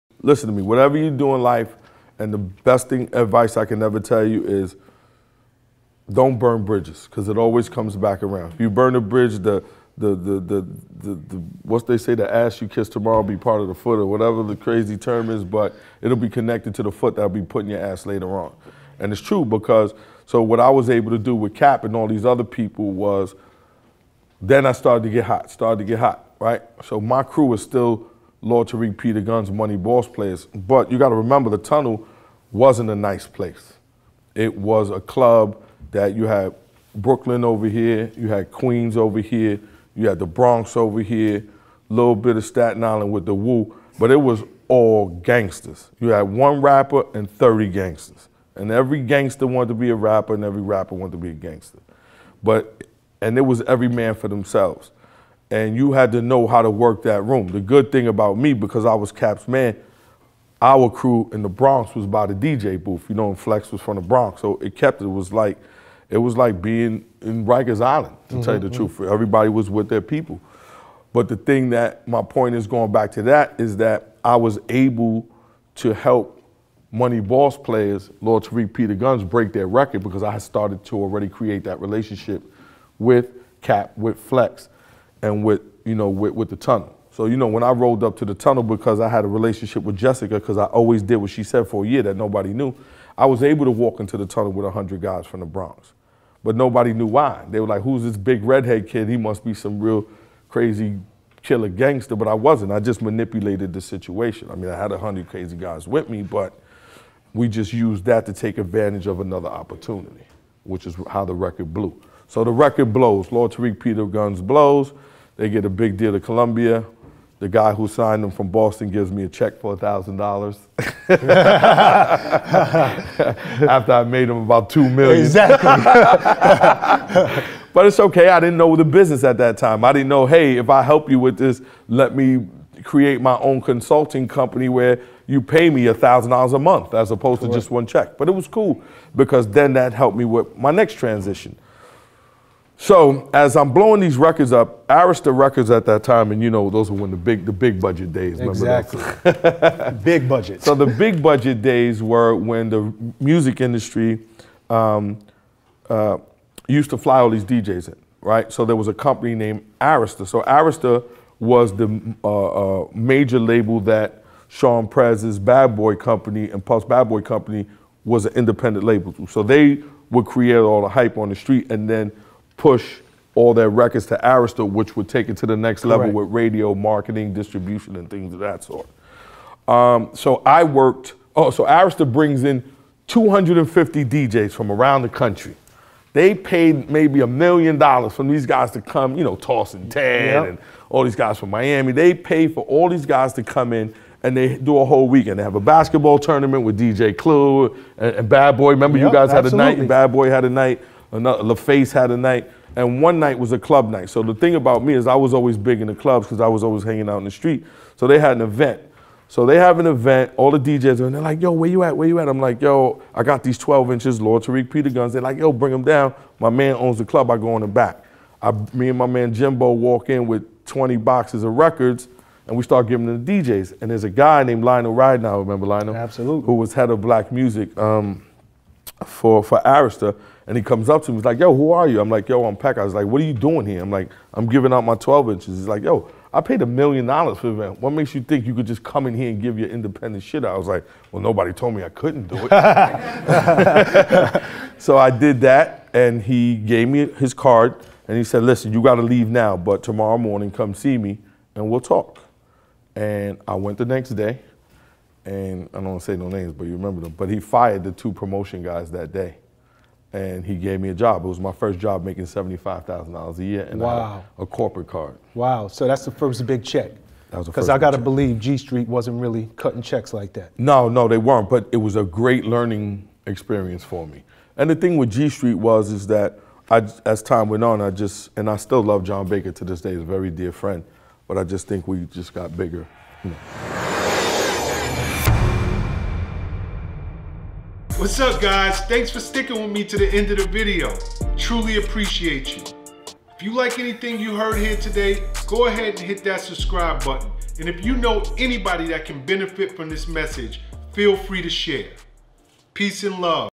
SHAWN "PECAS" COSTNER - "DON'T BURN BRIDGES"
PowerMove Maker, Shawn Pecas explains why it is imperative that you maintain good relationships. NEVER BURN A BRIDGE! The toes you step on today may be connected to the ass you'll kiss tomorrow!